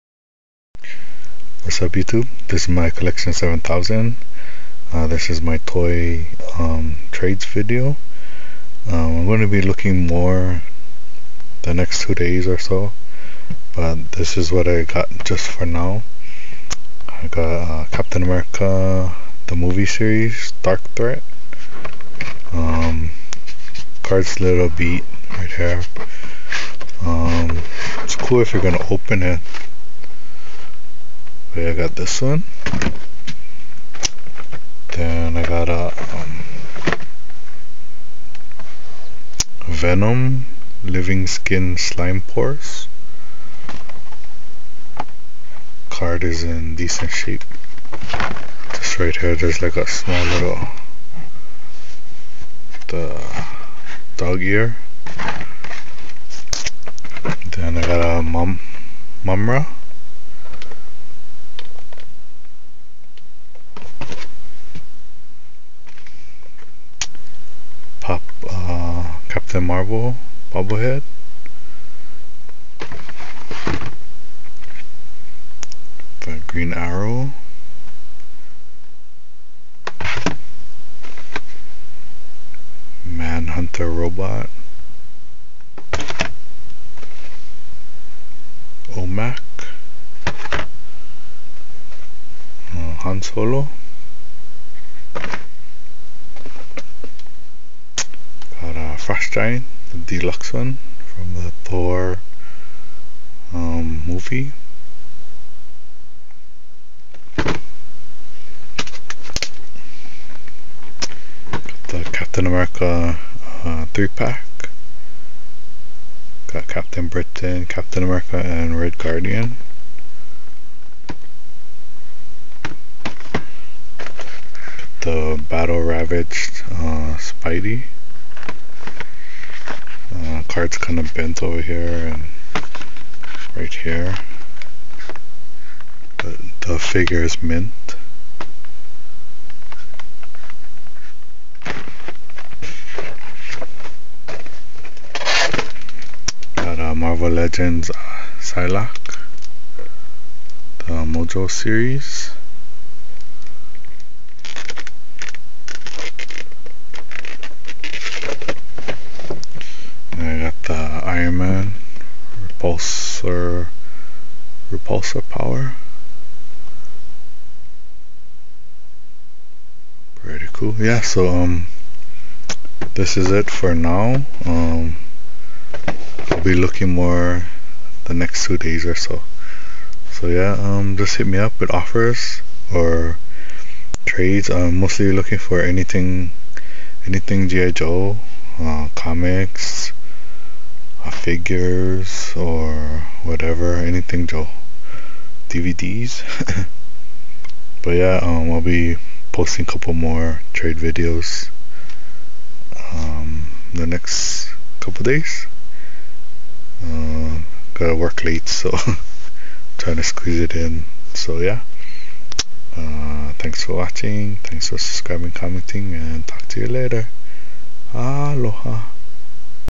mycollectionz7000's TOY TRADE video 7/15/12
here is just a few toys for trade for sun 7/15/12...i will be looking for more toys in the next couple of days..looking to trade anything gi joe-figures,comics,dvd,anything gi joe...will post a video of more stuff for trade..and as always thanks for watching and supporting my channel...BATTLE RAVAGED SPIDEY,MARVEL LEGENDS PSYLOCKE,VENOM,AND MUMM-RA HAS BEEN TRADED SO FAR